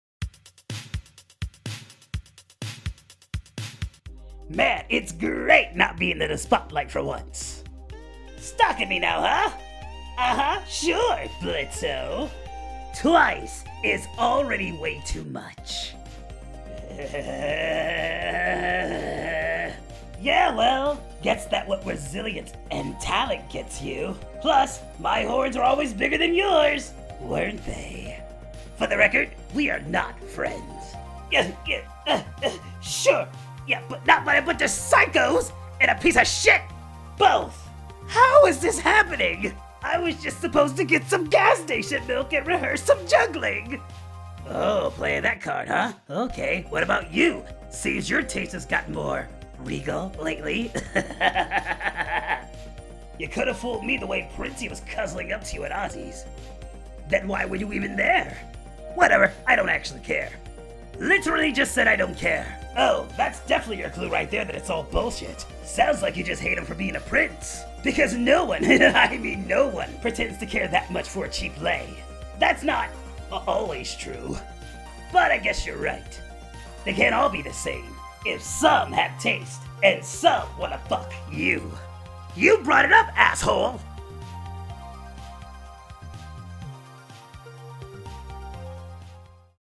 Fizzarolli 2 [Helluva Boss] | Impression
Did Another quick fun impression of fizzarolli from Helluva Boss.
Ugh hurt my throat doing multiple takes. lol